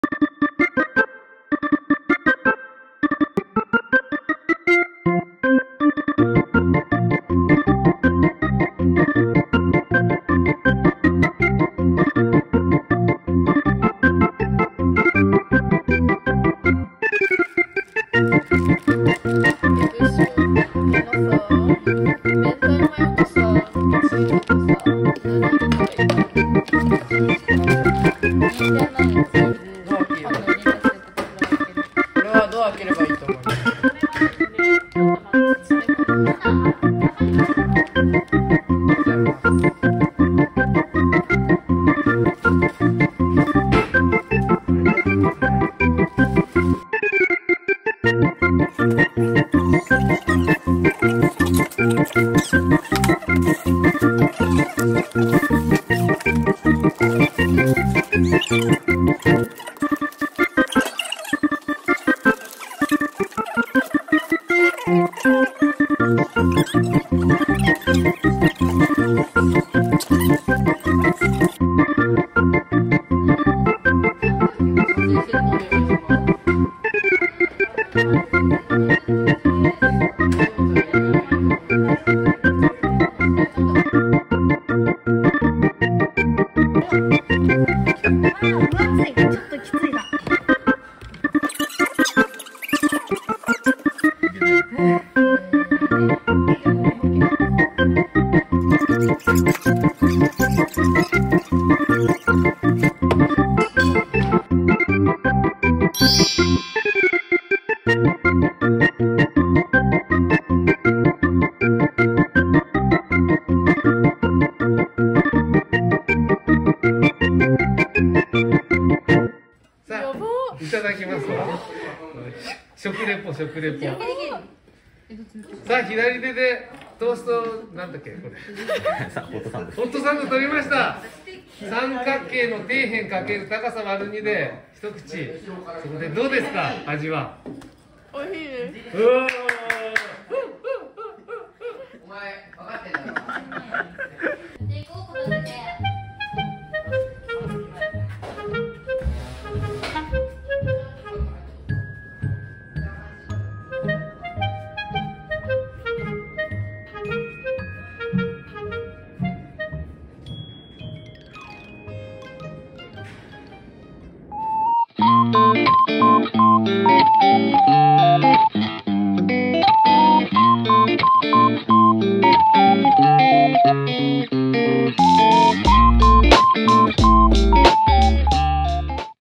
【中高生kitchen】ホットサンドを作ろう！【共育プラザ平井】
4月7日（日）に開催された中高生kitchenの様子を共育プラザ平井広報部の高校生が動画にしてくれました！
にぎやかな様子をぜひご覧ください。